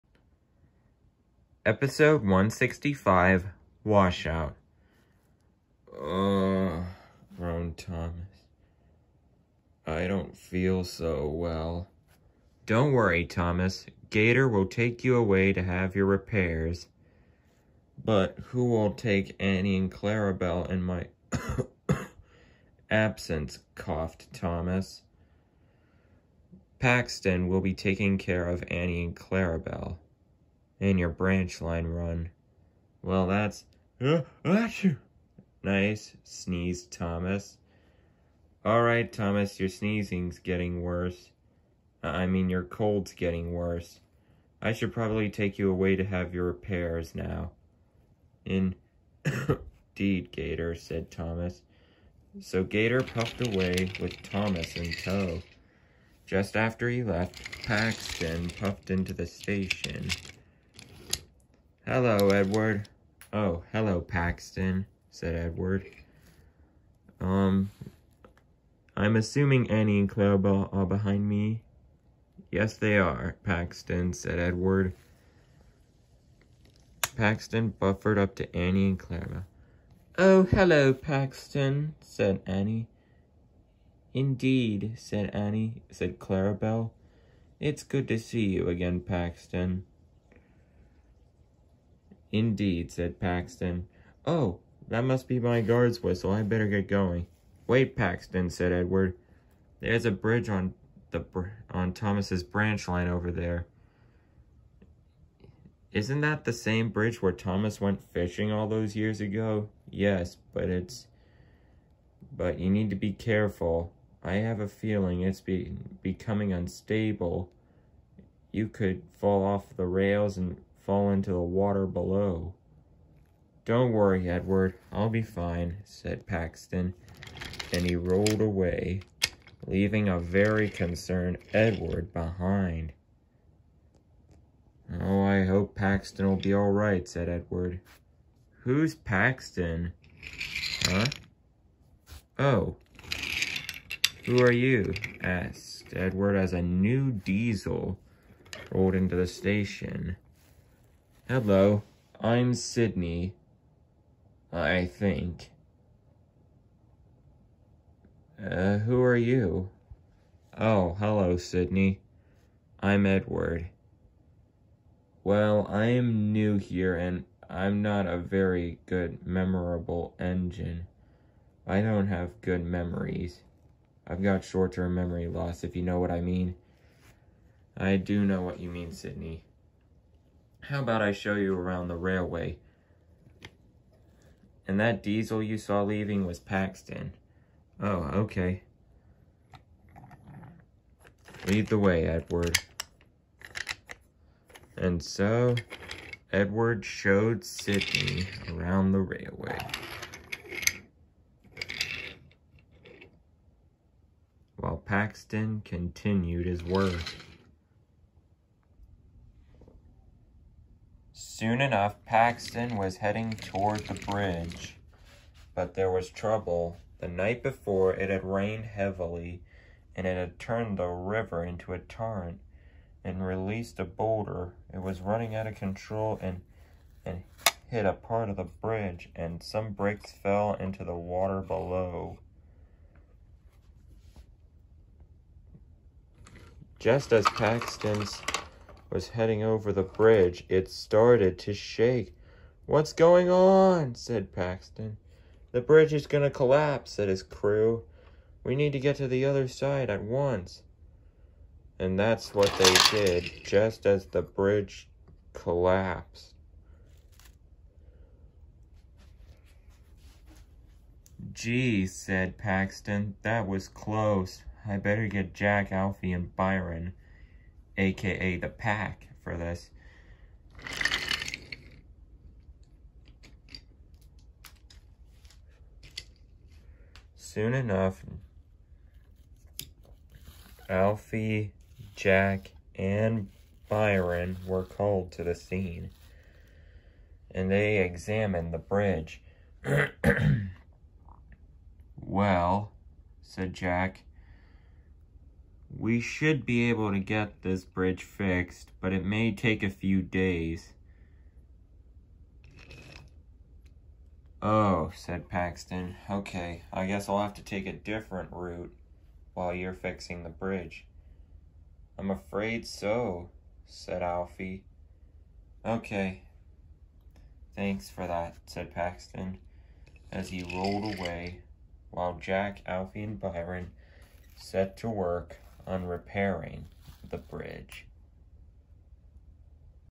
TWRATTTE&F Ep. 165 Washout 💦
Thomas is ill, and it is up to Paxton to do his branch line passenger run, will he be successful with the job at hand? Enjoy the fifth episode of season 17 of my series. Based off of “Washout” from Thomas’ Number 1.

Now introducing Sidney the Forgetful Diesel Engine to my TWRATTTE&F Series.

I also got a new video editing app to start using as well.